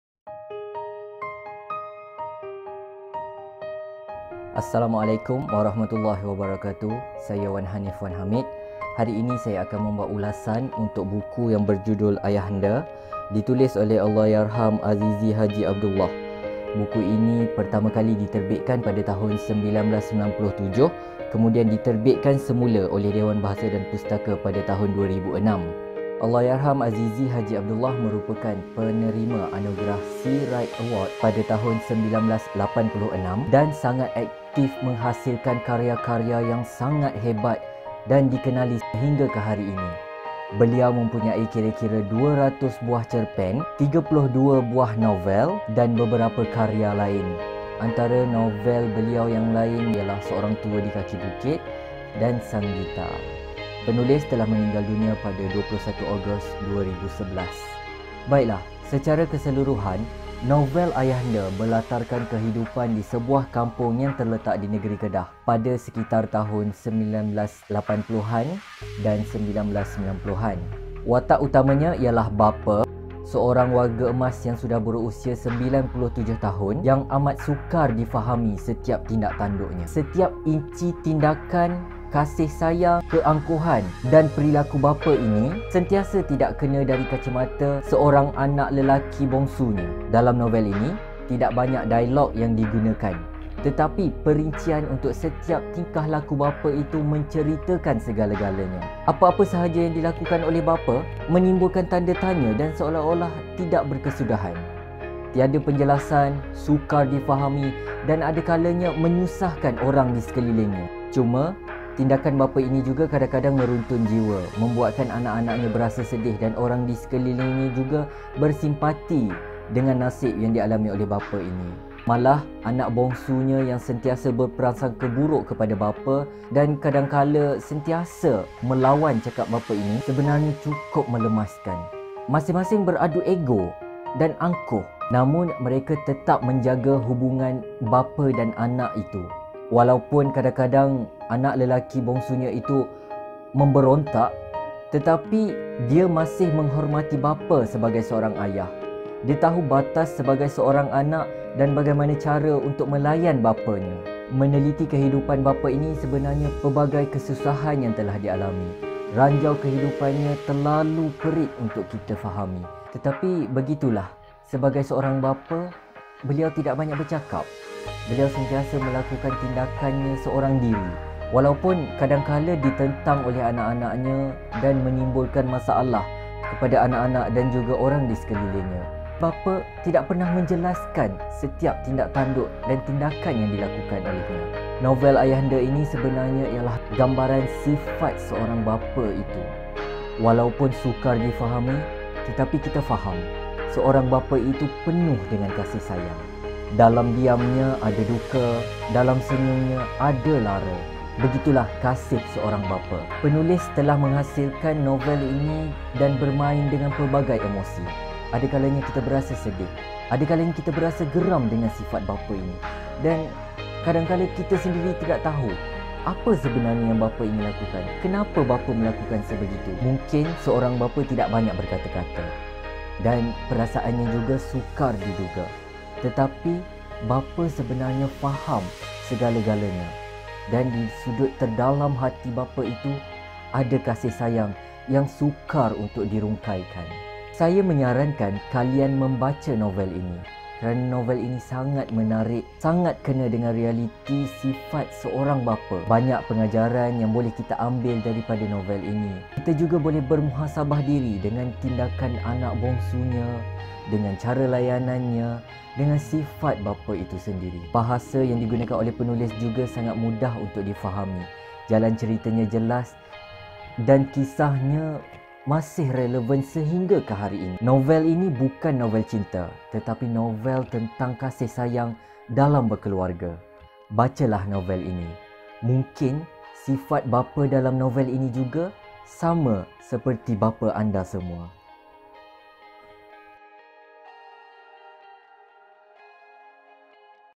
ULASAN NOVEL AYAHANDA KARYA AZIZI HAJI ABDULLAH
Ulasan novel Ayahanda karya Azizi Haji Abdullah, terbitan Dewan Bahasa dan Pustaka sempena Pertandingan Ulasan Buku Dekad Membaca Kebangsaan peringkat sektor awam 2020.